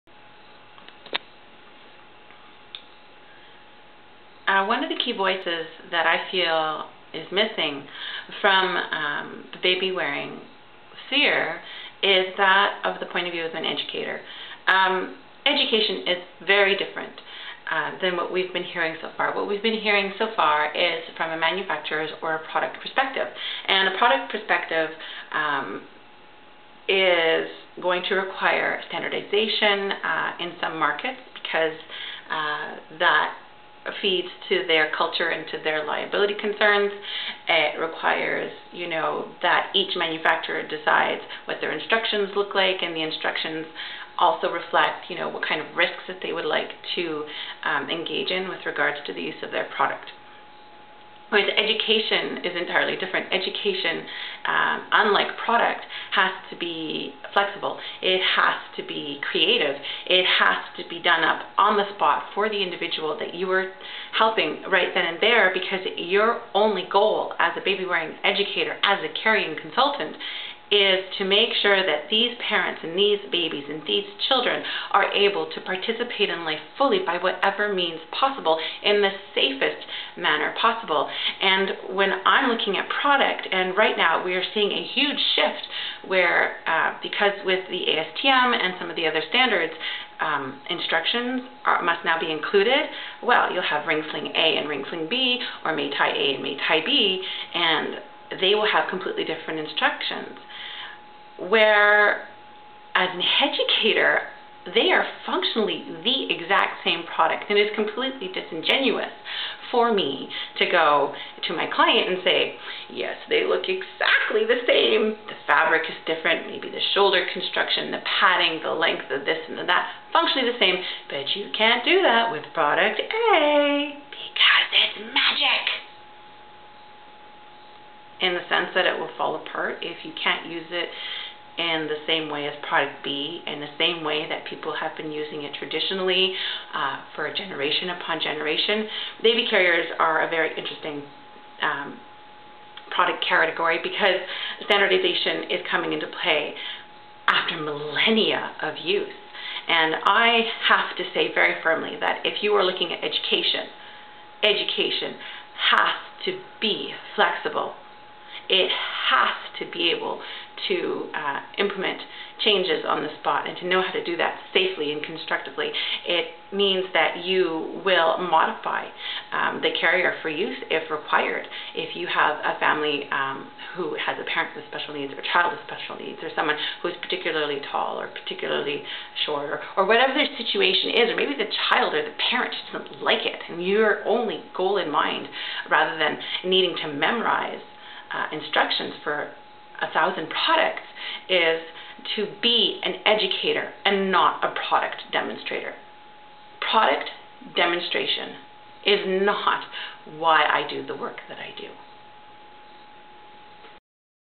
Flexibility in education is important
Product demonstration is not why I do the work as I do as an educator. It is to provide empowerment to the parents who seek me out by applying the knowledge I have accumulated over the course of my career.  My first goal isn't to sell new product.  It is not about memorizing all the carrier instructions so that if a client comes in with product A, whose instructions do not include well-known positions, I automatically recommend a new. It is to help them achieve perfection in this moment in time.